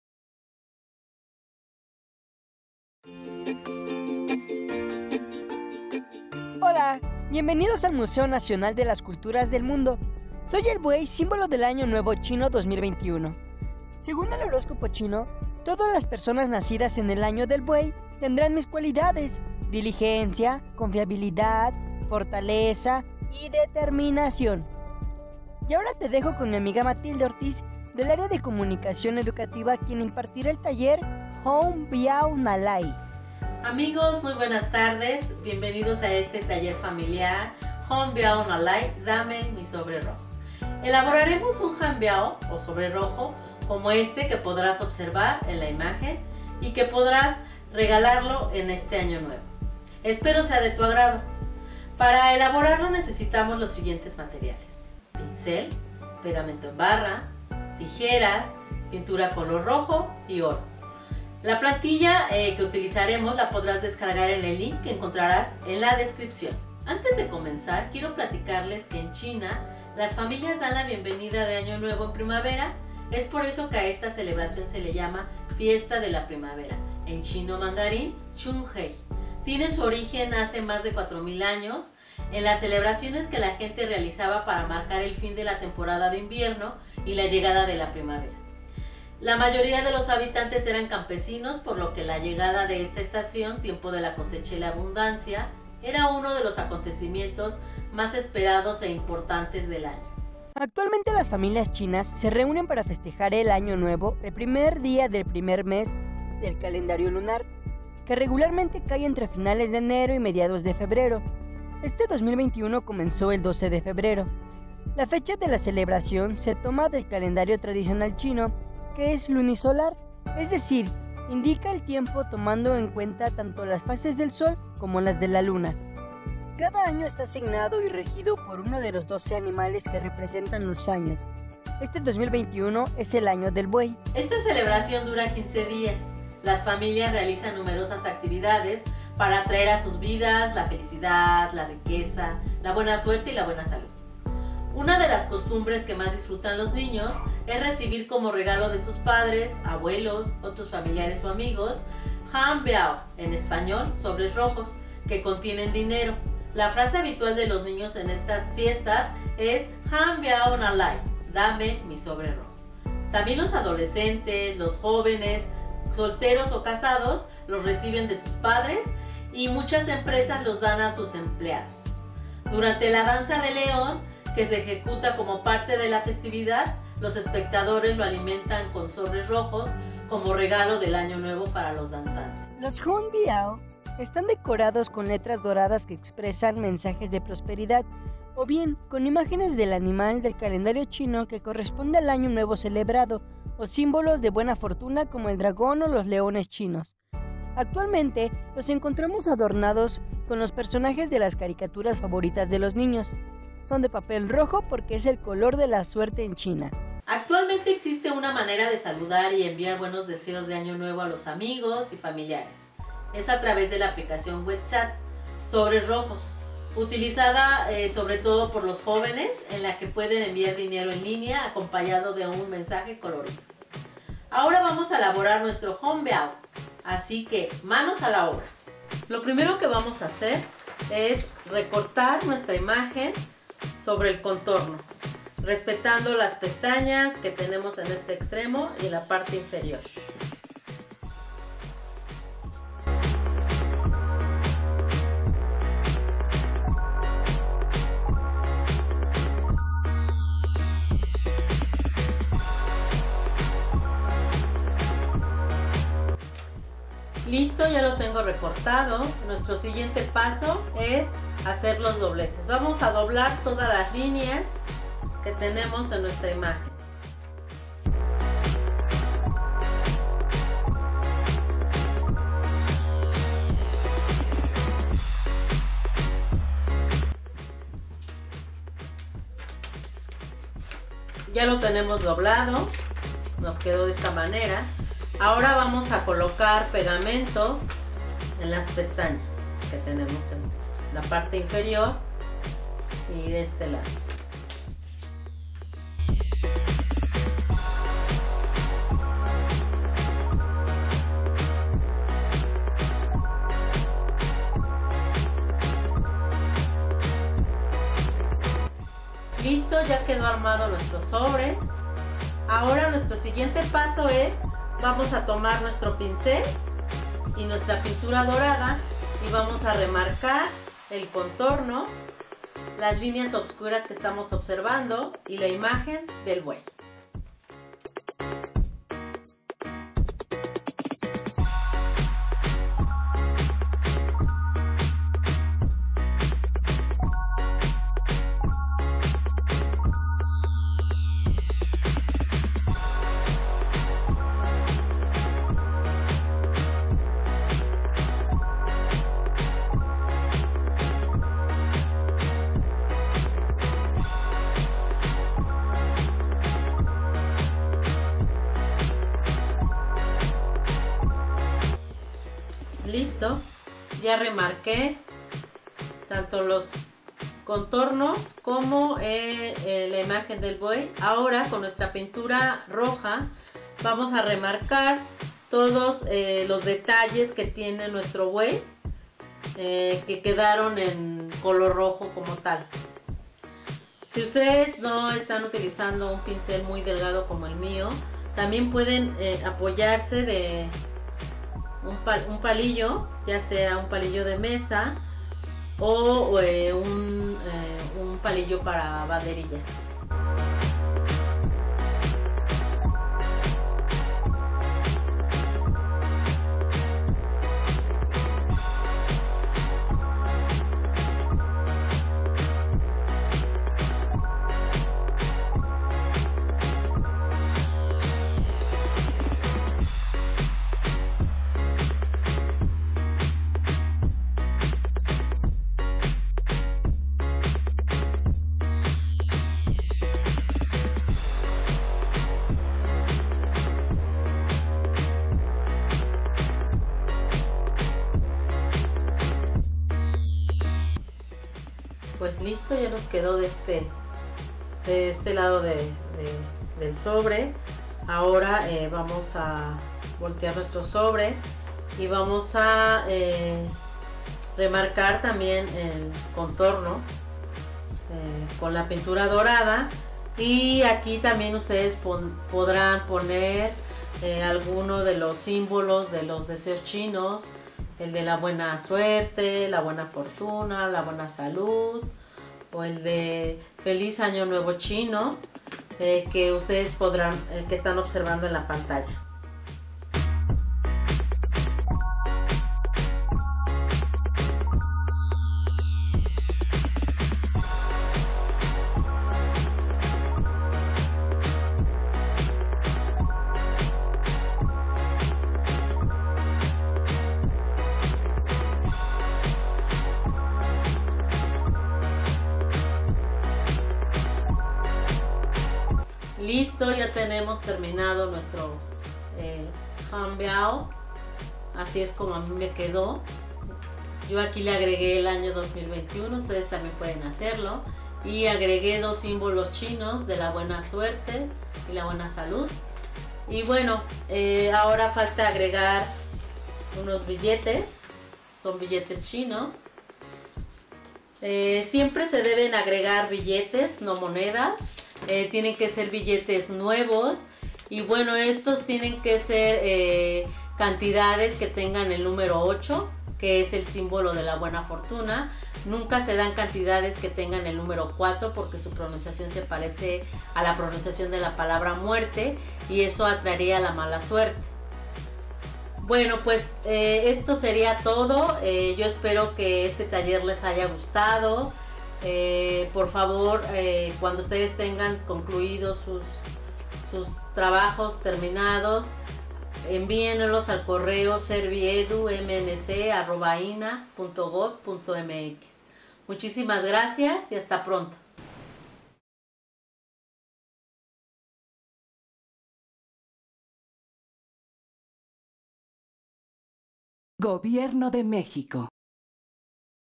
Taller familiar Dame mi sobre rojo - MNCM
En este video del Taller familiar en línea “Hóng Bao Ná Lái ¡Dame mi Sobre rojo!”, nuestra compañera Matilde Ortiz, de Comunicación Educativa del Museo, nos explica el origen de esta tradición y enseña a elabrorar un sobre para desear la prosperidad y la buena fortuna.

Descarga la lista de materiales y la plantilla correspondiente a esta actividad en el siguiente enlace
En caso de que se te indique que tu explorador se tiene que actualizar, te recomendamos cerrar la ventana, copia mejor el enlace y pégalo en una nueva ventana de tu navegador de Internet.
Ahí encontraras todos los archivos (en formato PDF) con las plantillas y la lista de materiales necesario.